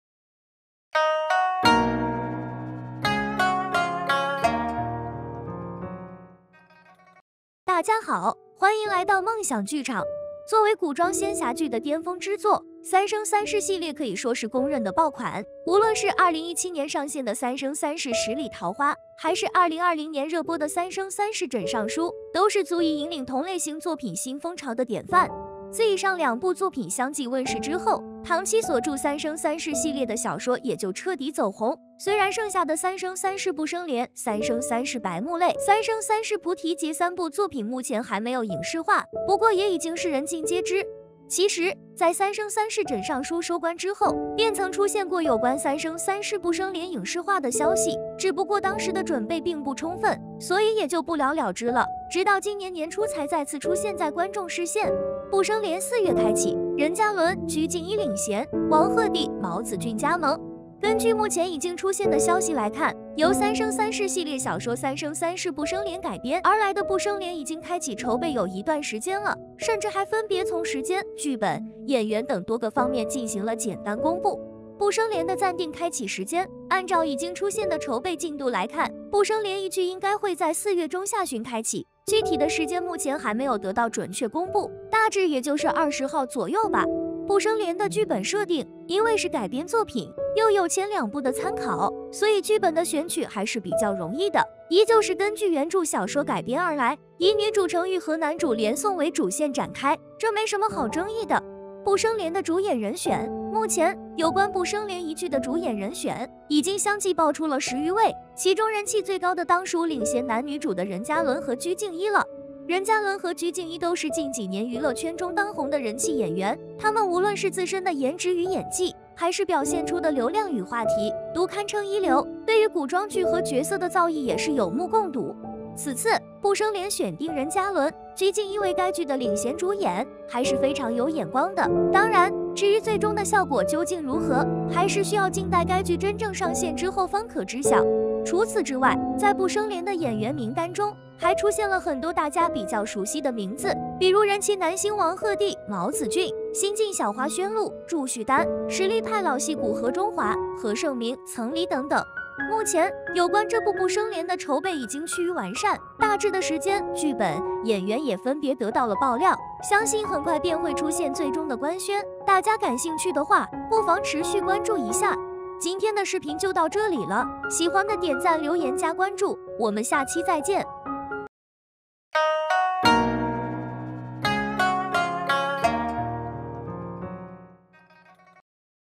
《步生蓮》四月開啟，任嘉倫、鞠婧祎領銜，王鶴棣、茅子俊加盟！
《步生蓮》四月開啟，任嘉倫、鞠婧祎領銜，王鶴棣、茅子俊加盟！任嘉倫